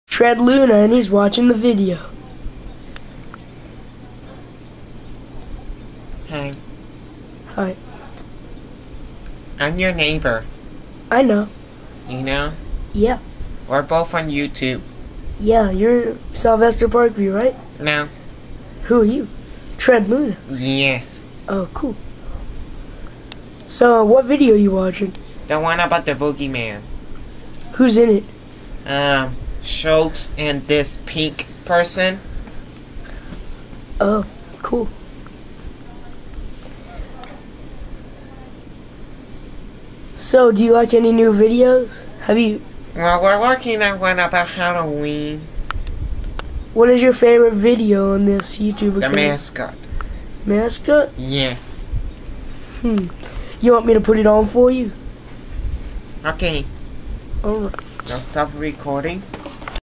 Tred Luna on the computer, Taylor Ellis
Tred Luna visits me ,Taylor Ellis. Puppeteer, Samuel Santiago. Filmed by, Taylor Ellis.